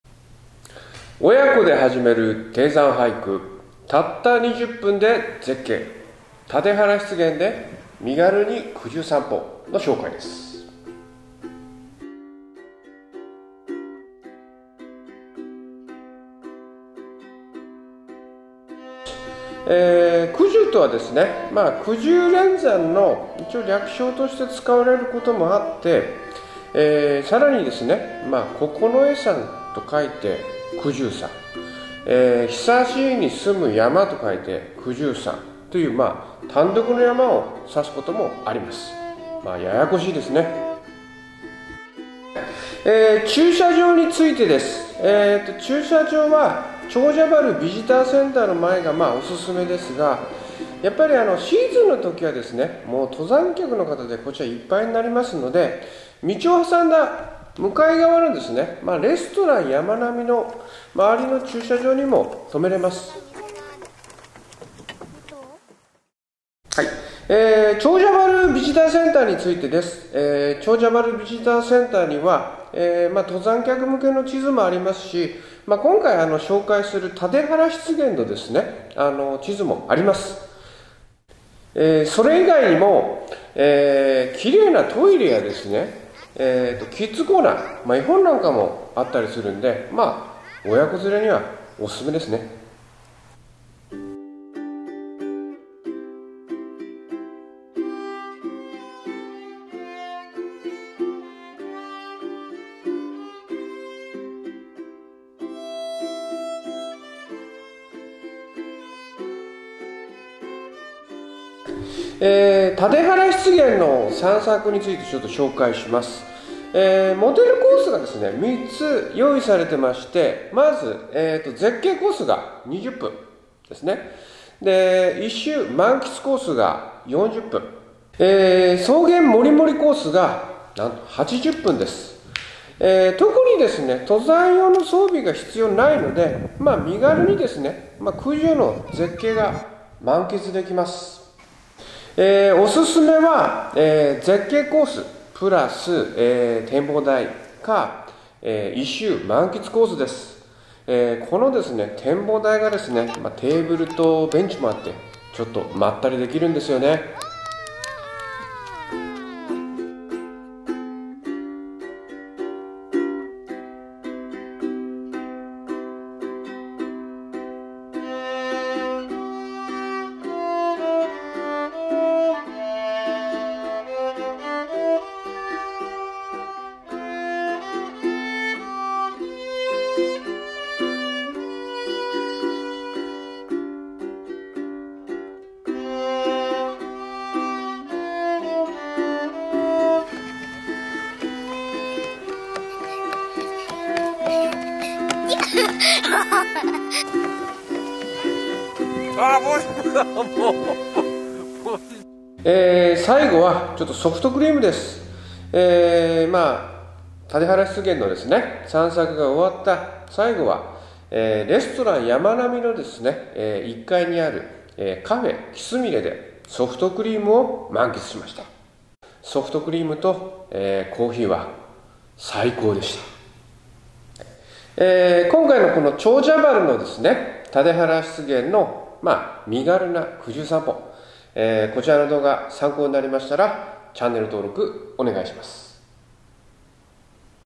【くじゅう散歩】長者原を身軽に山歩き！大分県玖珠郡九重町のタデ原湿原はたった20分で絶景！親子ではじめる低山ハイク
絶好の紅葉シーズンに大分県玖珠郡九重町の「タデ原湿原」で、
絶景の紅葉に萌えるくじゅう連山を満喫してきました。

長者原ビジターセンターを起点にたった20分でも絶景を堪能できるお手軽で、
タデ原湿原だけだったら登山装備も必要ないので身軽に堪能できるおすすめスポットです。

低山ハイク、お出かけスポットとしていかがでしょうか。

↓もう少し地図なんか詳細のURLを記載したブログはコチラです。↓